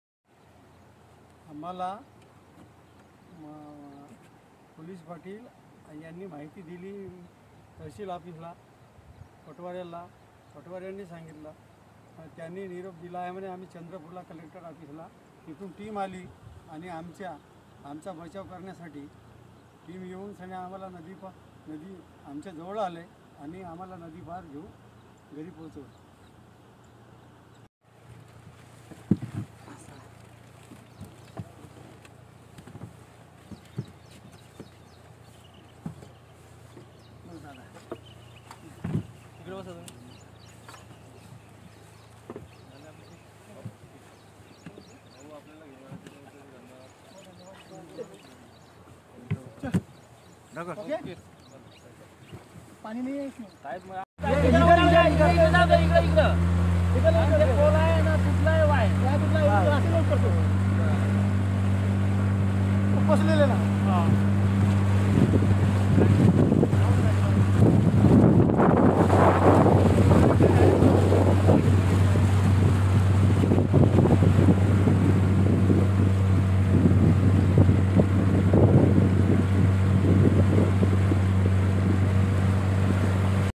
CHANDRAPUR | भात रोवणीसाठी शेतात गेलेल्या पाच शेतकऱ्यांची आज 24 तासानंतर पुरातून सुखरूप सुटका
चंद्रपूर जिल्ह्यातील चिमूर तालुक्यात सध्या पुराने थैमान घातले आहे. याच तालुक्यातील खतोडा येथील पाच शेतकरी ट्रॅक्टरने शेतात गेले होते. भात रोवणीची कामे करीत असतानाच काल दुपारनंतर पुराचे पाणी अचानक शेतात वाढले. चारीबाजूने पाण्याने वेढा दिल्याने घाबरलेले हे शेतकरी जीव वाचवण्यासाठी ट्रॅक्टरच्या टपावर आणि लगतच्या झाडावर चढून बसले.  कुणीतरी मदतीला येईल, या आशेने त्यांनी जागच्याजागी रात्र काढली.  रात्रभर घरची माणसे आली नसल्याने याची माहिती तहसीलदारांना देण्यात आली. बचाव पथकाला चंद्रपुरातून पाचारण करण्यात आले. हे पथक  पोचल्यानंतर बचावकार्य सुरू झाले. घटनास्थळी हे पाचही जण जीव मुठीत घेऊन बसले होते. या सर्वांना सुखरूप बाहेर काढण्यात आले.